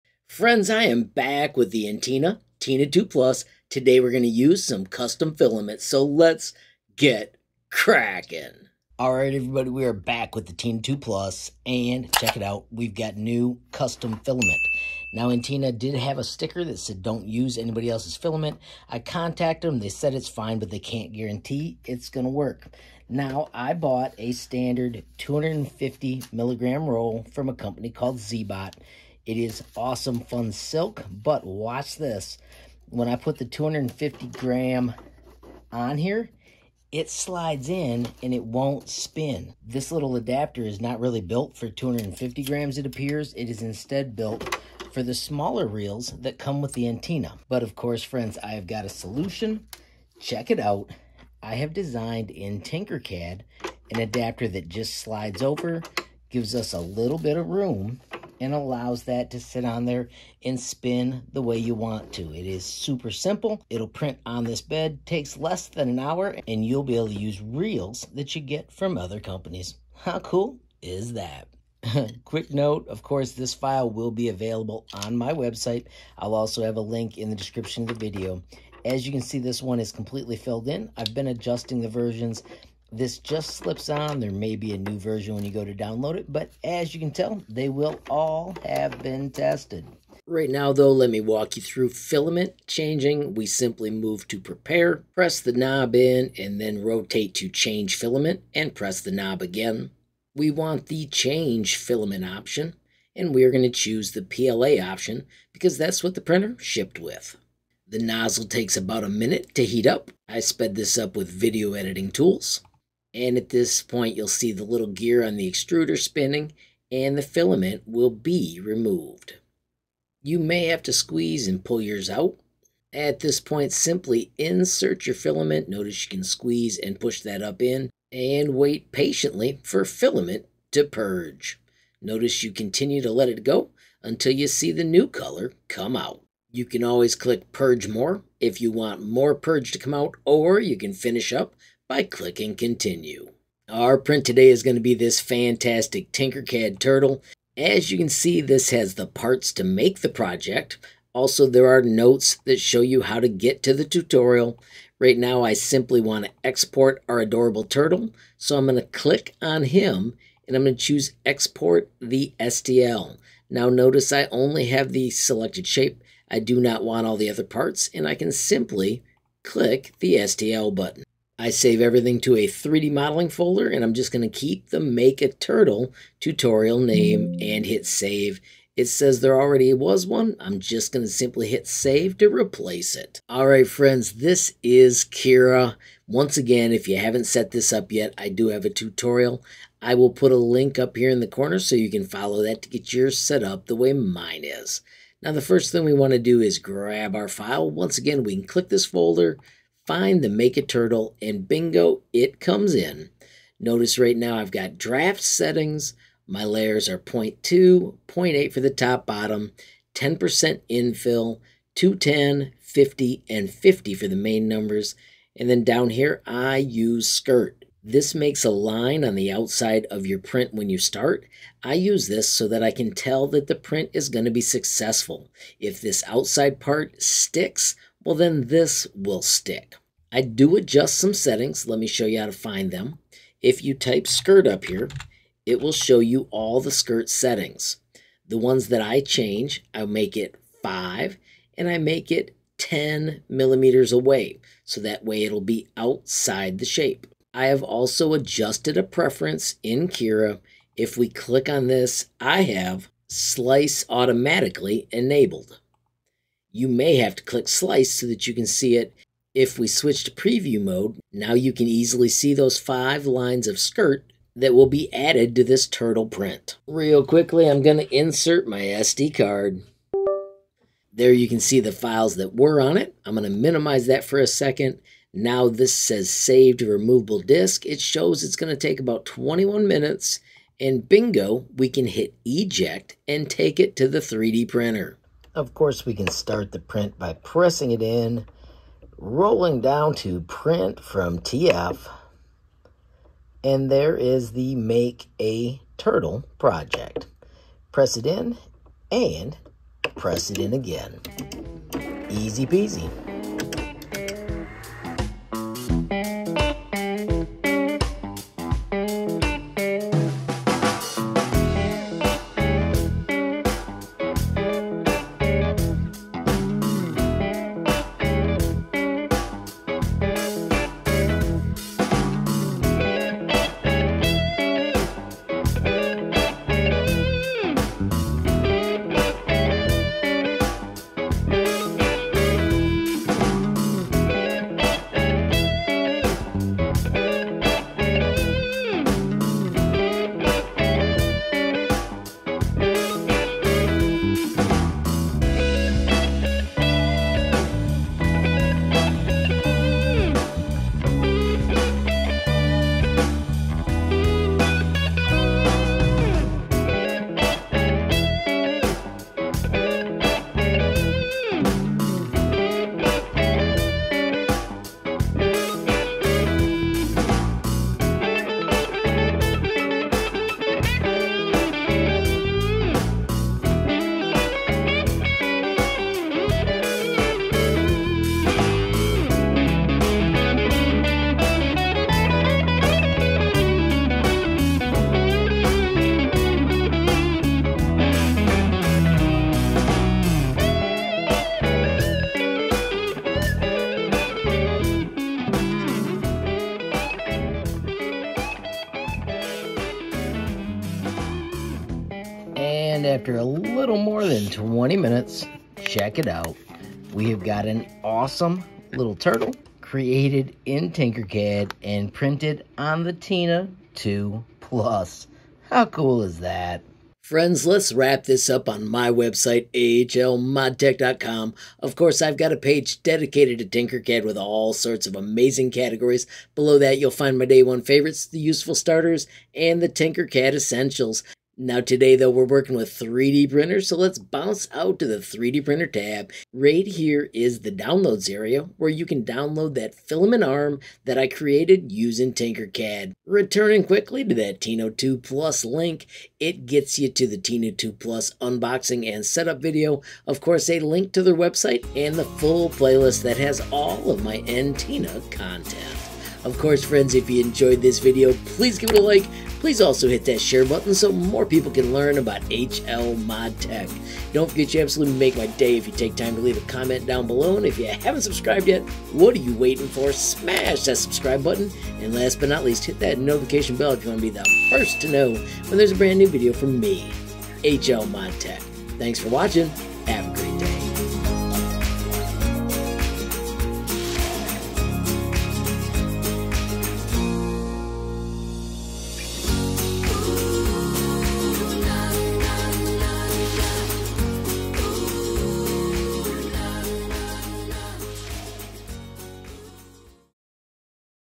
Free Tinkercad Filament Adapter & Generic PLA Entina Tina2 Plus Tips
A Free Tinkercad Filament Adapter & Generic PLA Tips Entina Tina2 Plus 3dprinter The adapter makes it easier to work with 250G filament reels.